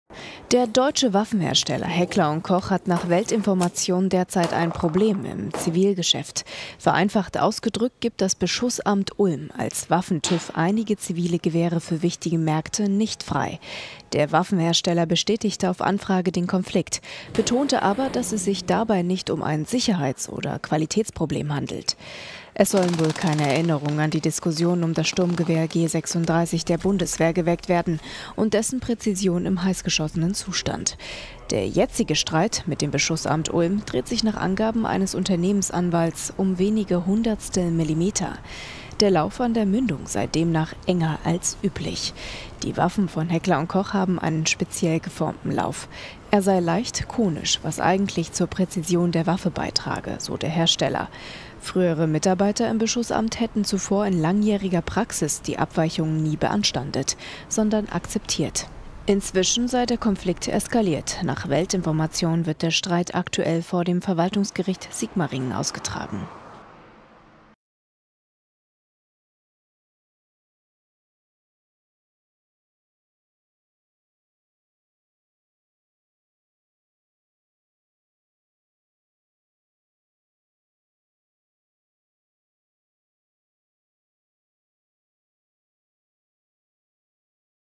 HECKLER & KOCH: Warum der "Waffen-TÜV" Gewehren die Zulassung verweigert
Der Waffenhersteller Heckler & Koch hat ein Problem. Das Beschussamt in Ulm verweigert einigen Militärsturmgewehren für Zivilpersonen die Zulassung. Der Lauf an der Mündung sei enger als üblich.